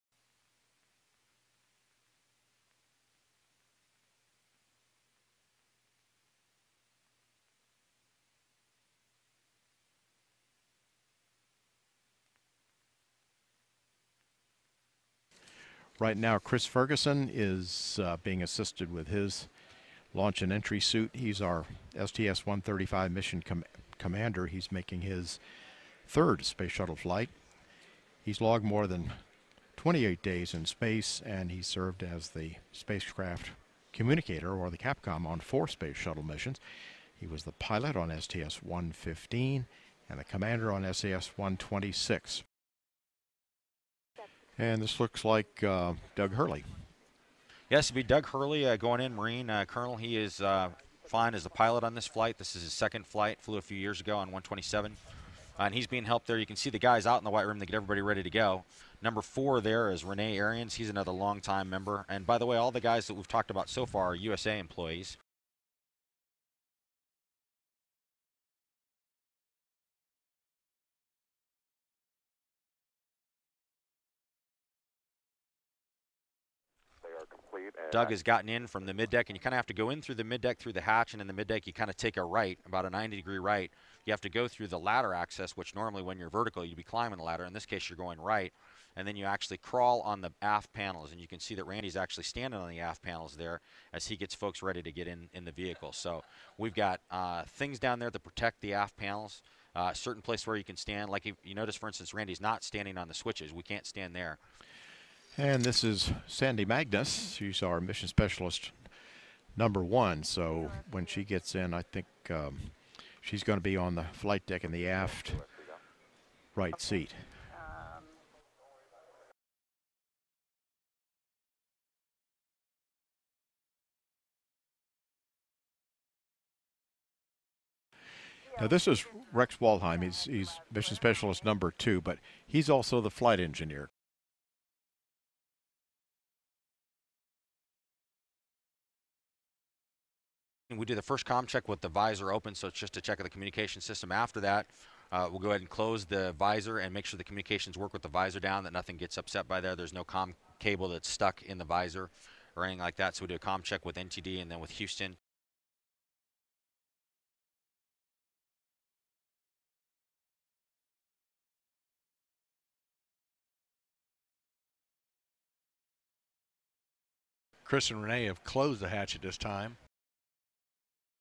STS-135 Astronauts Strap into Space Shuttle Atlantis for Launch
The astronauts enter Atlantis to be strapped in for their flight.